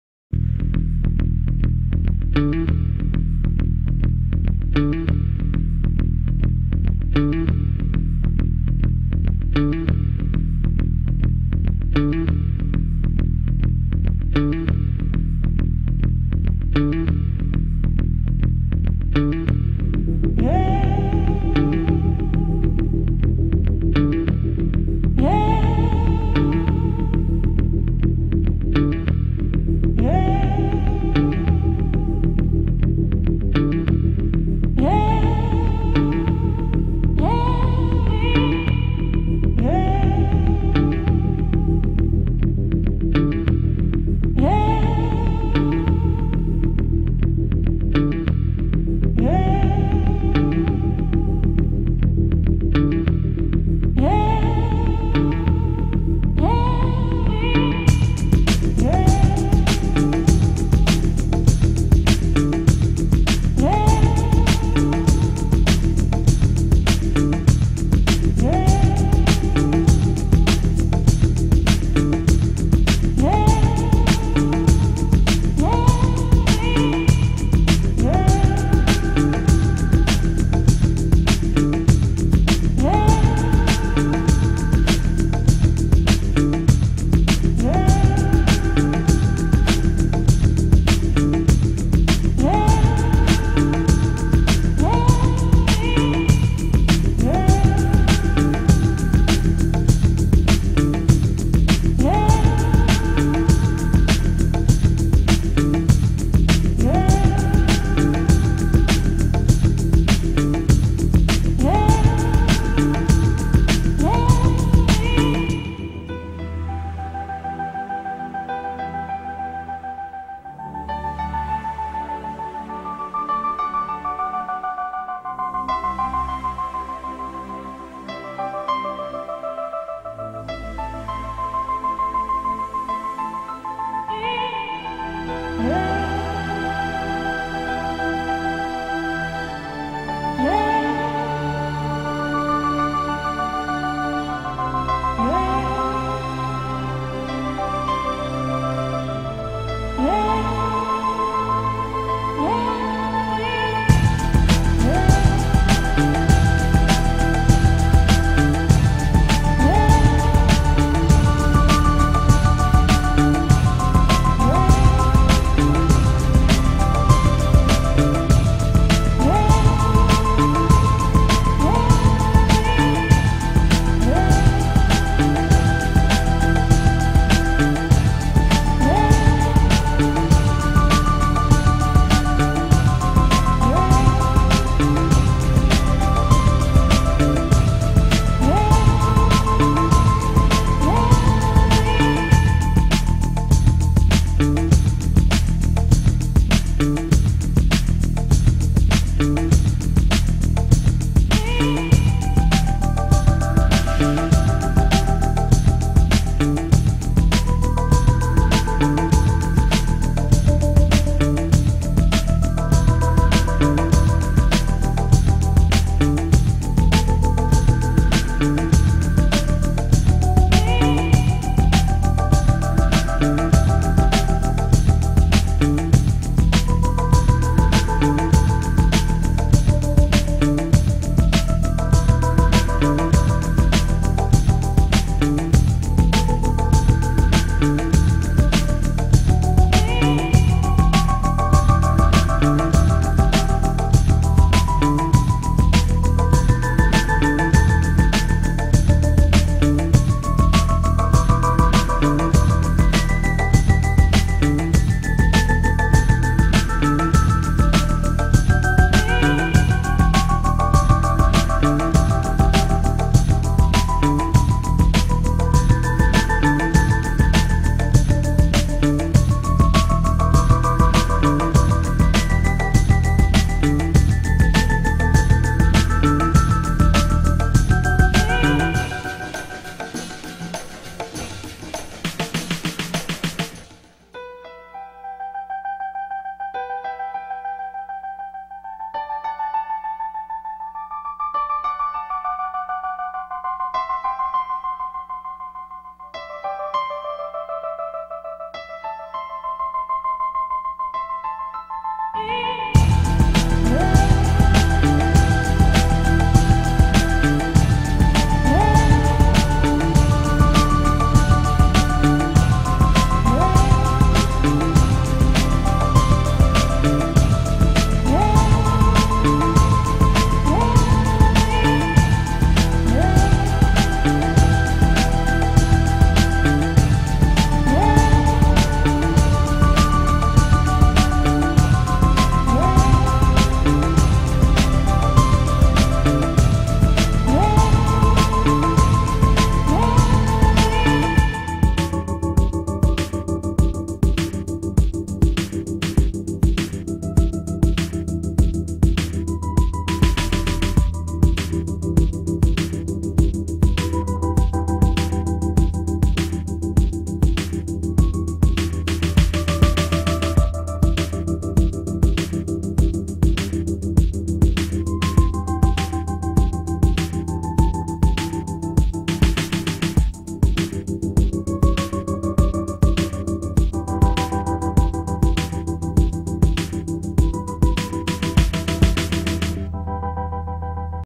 Solar flares and other starry stuff (long Moby version)
SOHO LASCO C2 images. (Last 1500)

very sorry for the screencast logo.

Watch the comet hit at:  4.35